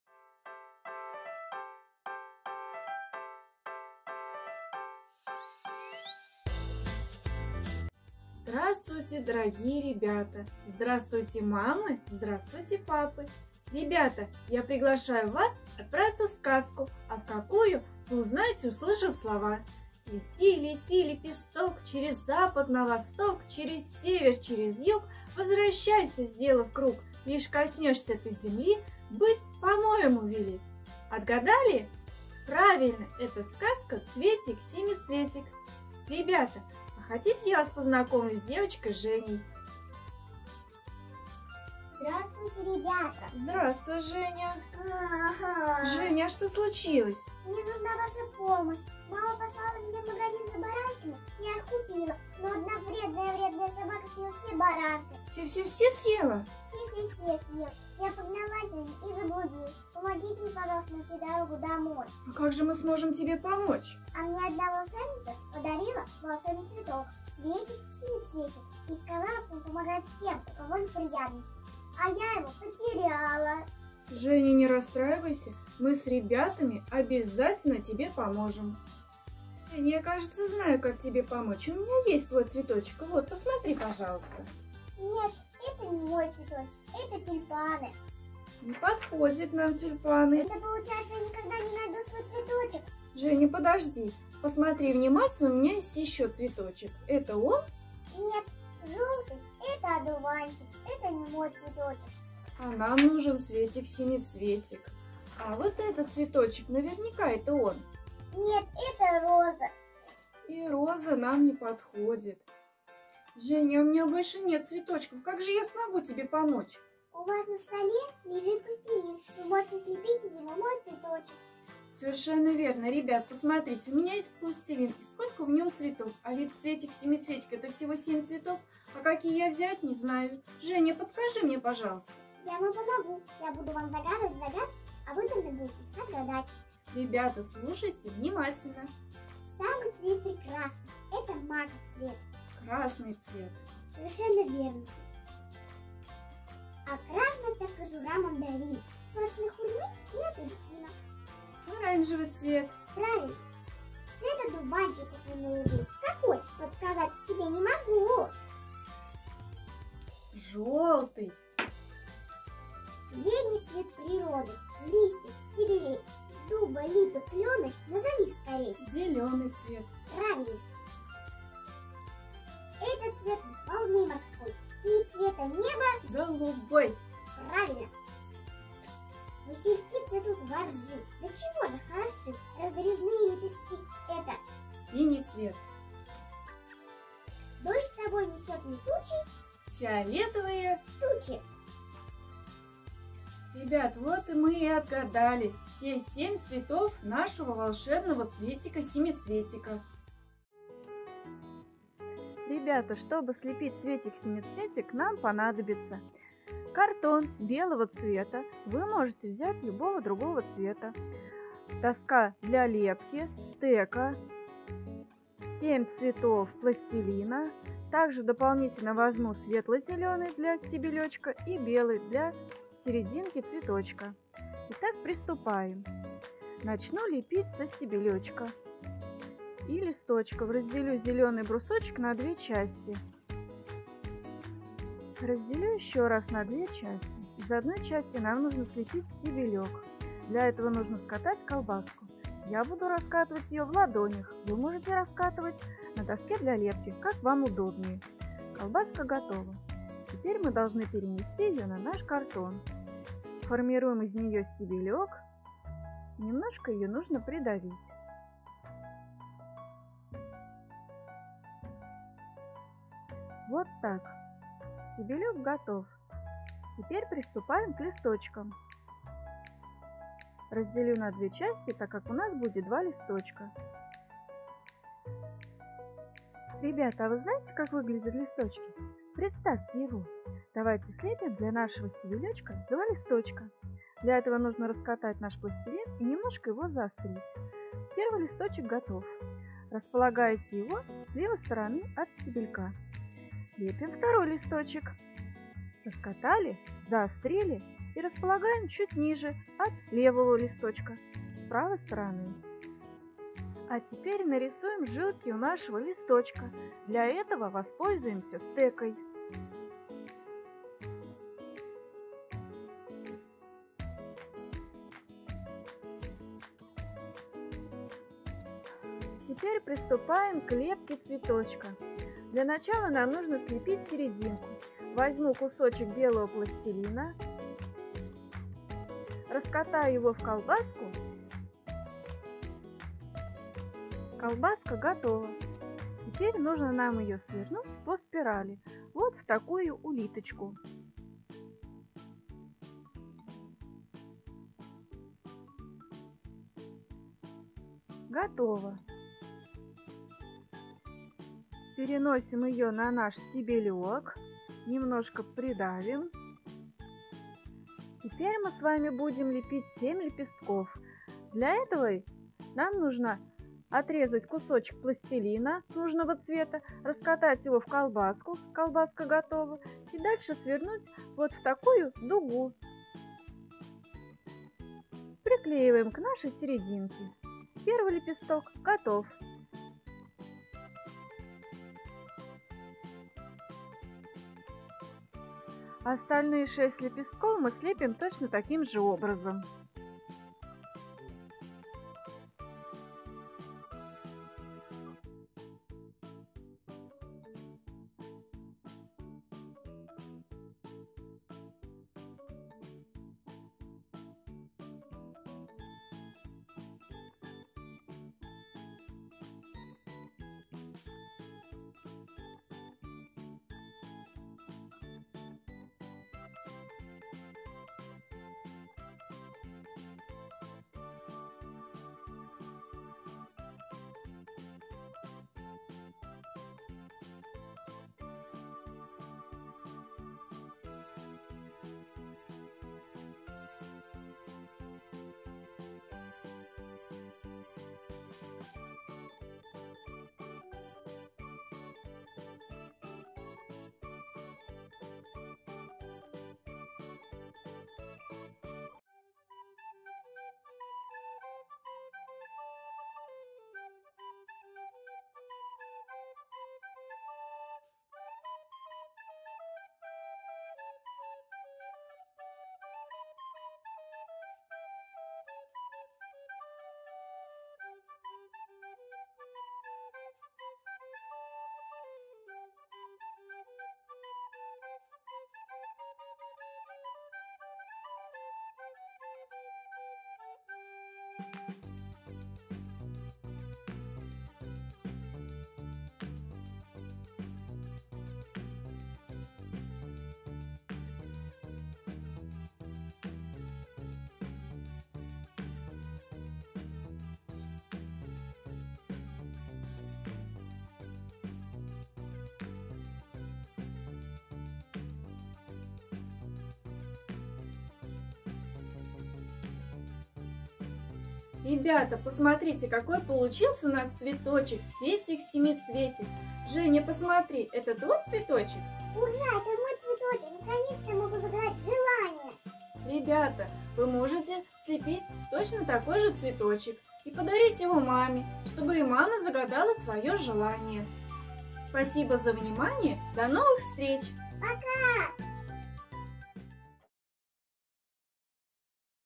Лепим из пластилина Цветик Семицветик
Мы с Вами отправимся в путешествие в сказку Цветик-Семицветик, в которой познакомимся с девочкой Женей. Поможем ей найти её цветочек, а для этого нам нужно вспомнить название цветов и отгадать загадки! Ну и конечно слепить Цветик-Семицветик!